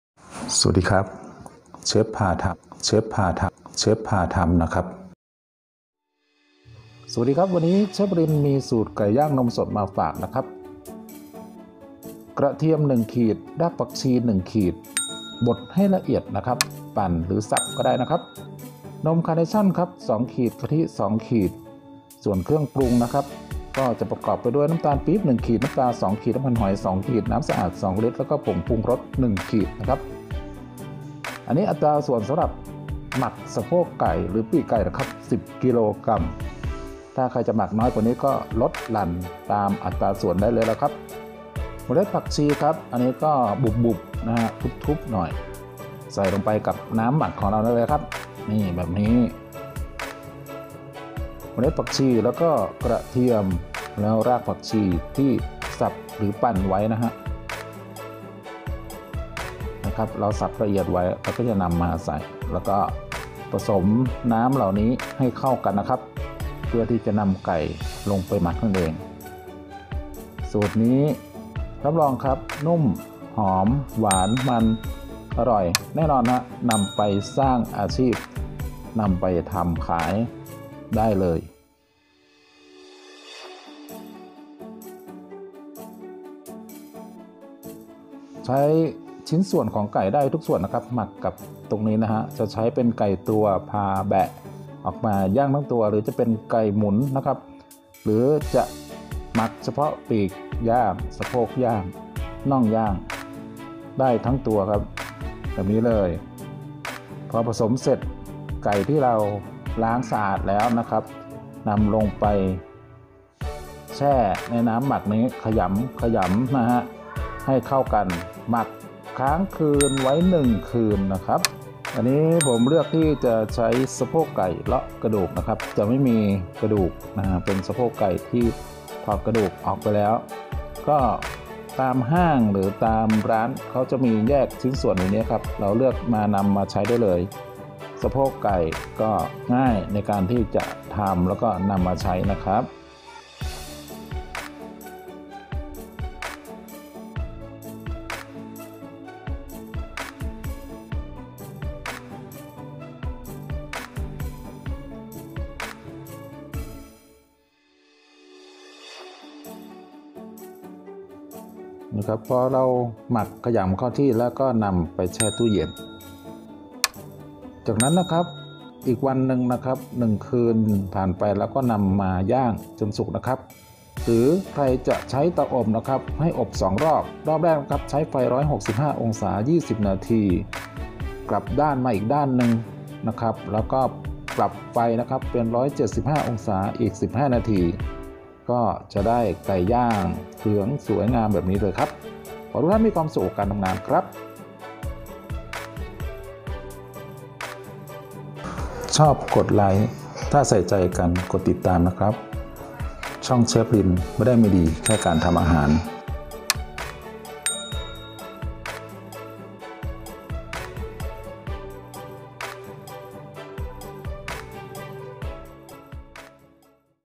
ไก่ย่างนมสด สูตรสร้างอาชีพ by Chefrin
ไก่ย่างนมสด
ส่วนผสม
ไก่  10  กก.
กะทิ  2  ขีด
นมคาร์เนชั่น  2  ขีด